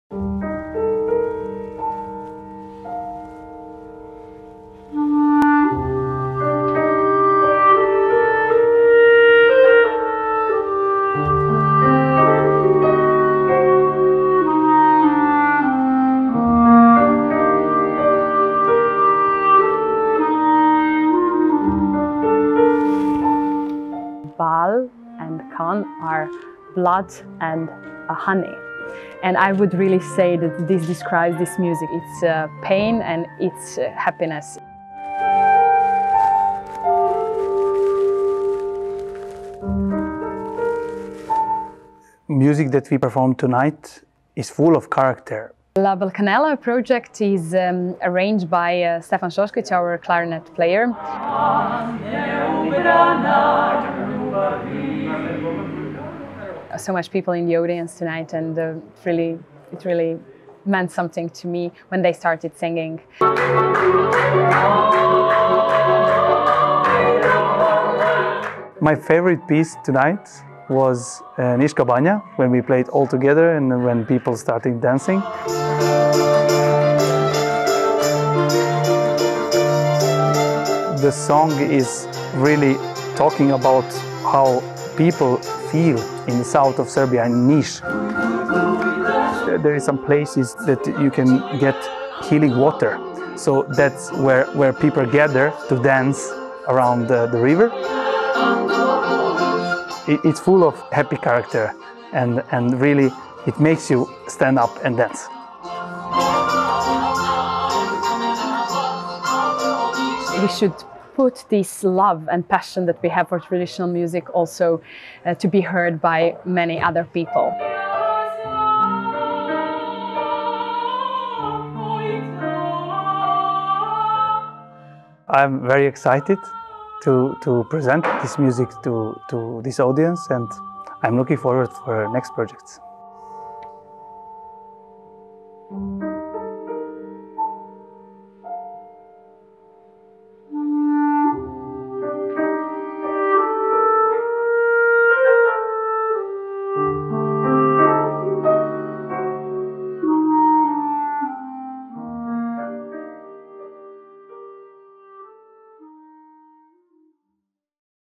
La Balkanella & Ethno Norway
La Balkanella 2021!

En rytmisk og spektakulær opplevelse fylt med rytmer og tradisjonellmusikk fra Balkan.

Olivera Ticevic, internasjonalt kjent sopran, Stefan Soskic (klarinett), Jelena Golubovic(piano) og Dejan Milivojevic på trekkspill, tar publikum på en uforglemmelig reise gjennom lang og rik musikkhistorie fra Balkan. Spilt under påvirkning av jazz og klassisk musikk gjør denne konserten særlig kompleks og i høy kvalitet